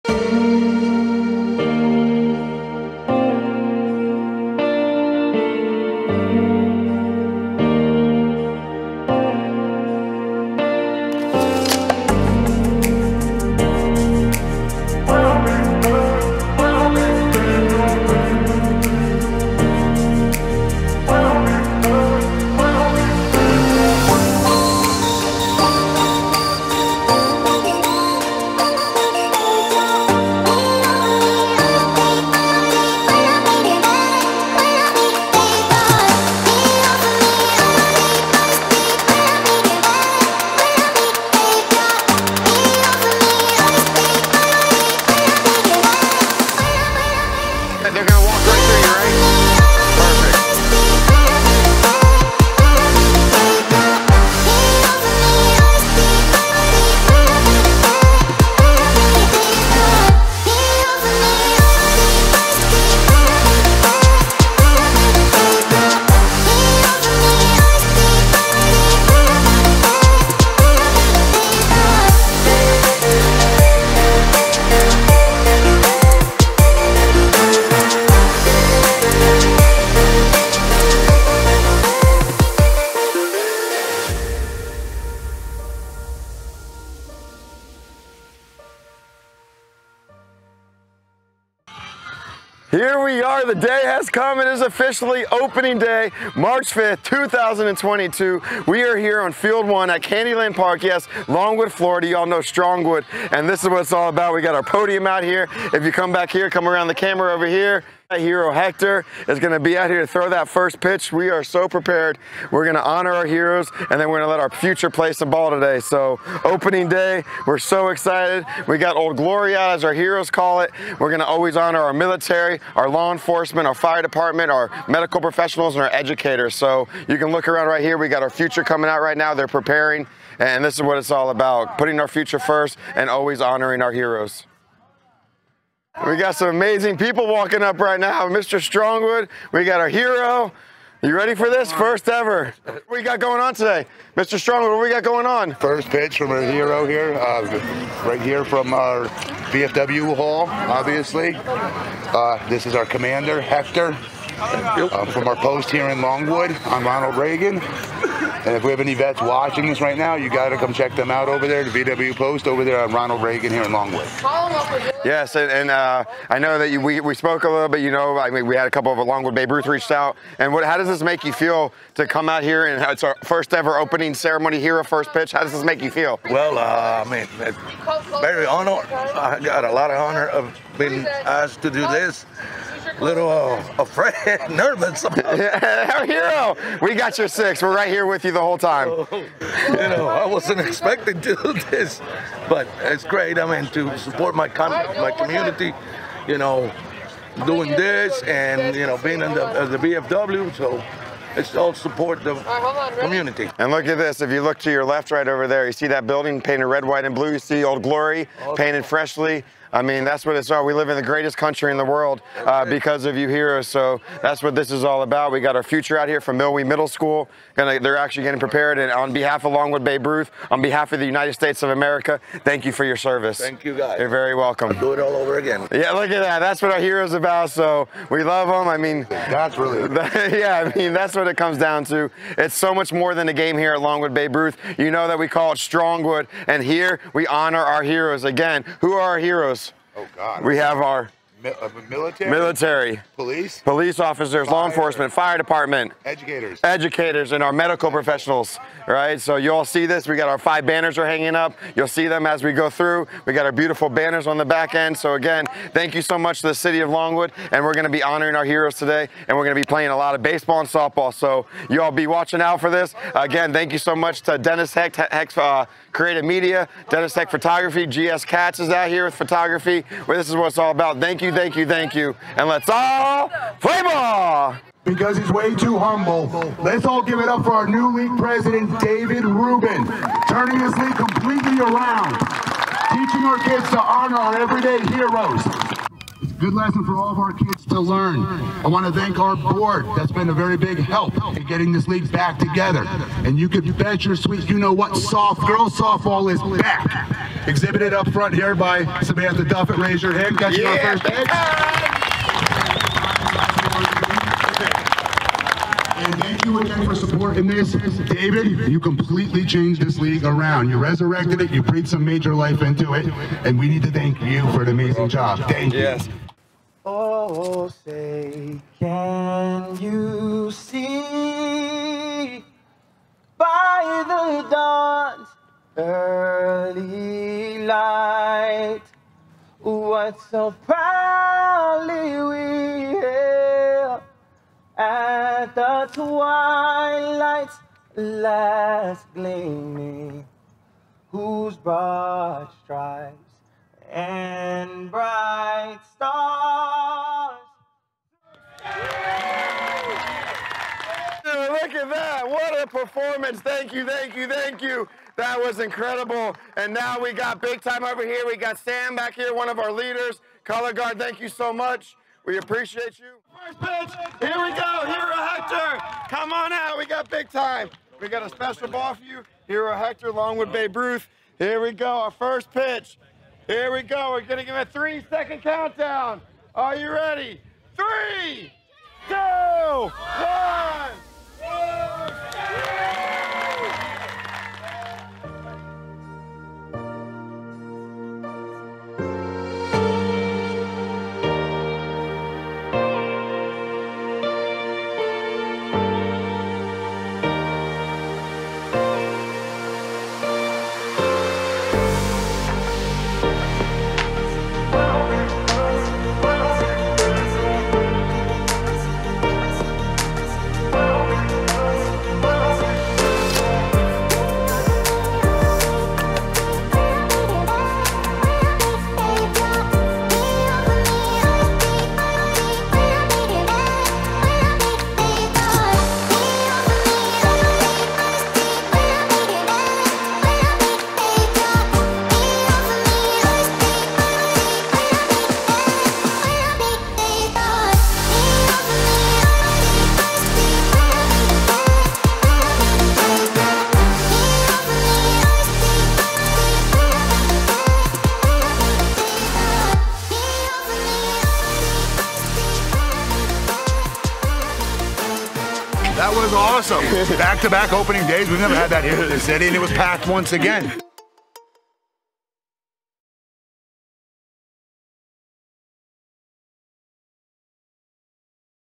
Longwood Babe Ruth Opening Day 2022
Longwood Babe Ruth welcomes unprecedented growth as the social media campaign led by Dennis Hecht of Dennis Hecht Photography / Hecht Creative Media has reached every corner of Seminole County.